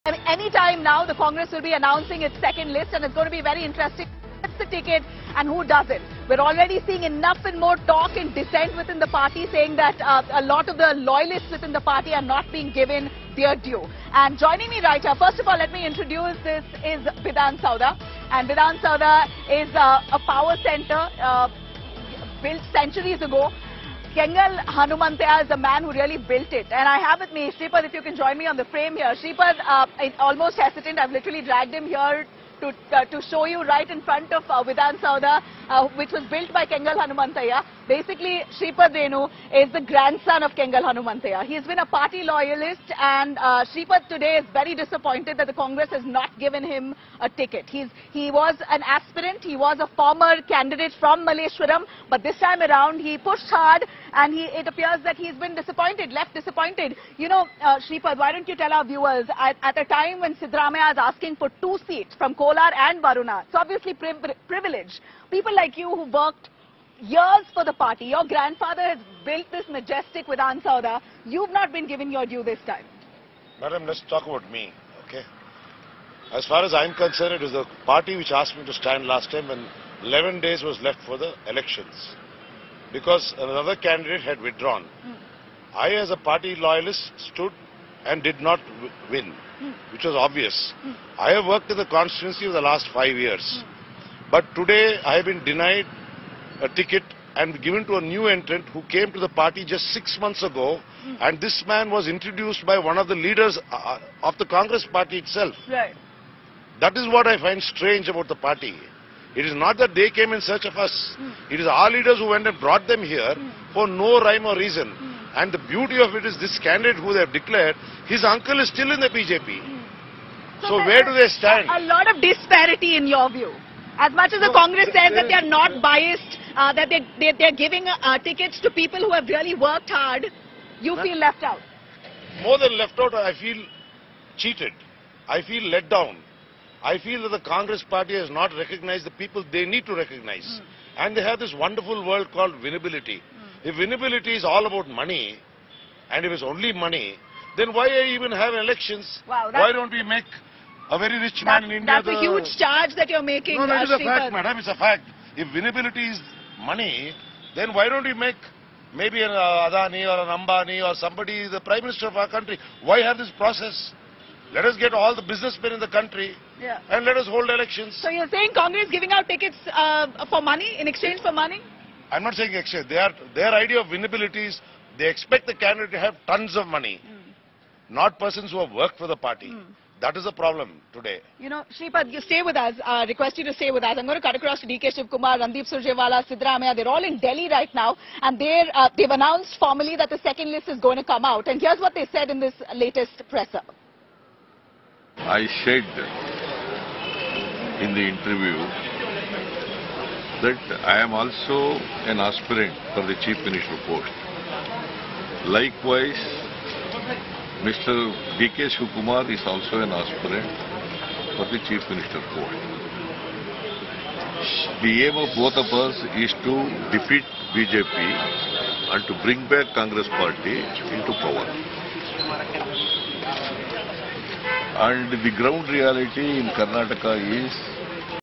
Cong's Kengal Shreepada Renu Said After Not Getting Ticket For Assembly Polls 2023: 'Feel Cheated'
State Of War Karnataka With Nabila Jamal: Congress To Announce Second List | Karnataka Election 2023

The Congress is likely to release its second list of candidates for the upcoming Karnataka Assembly election on Tuesday following a meeting of the party's Central Election Committee. The meeting will take place at 10.30 am at the Congress headquarters in Delhi.

Elections to the 224-member Karnataka Assembly will take place on May 10 and the results will be announced on May 13.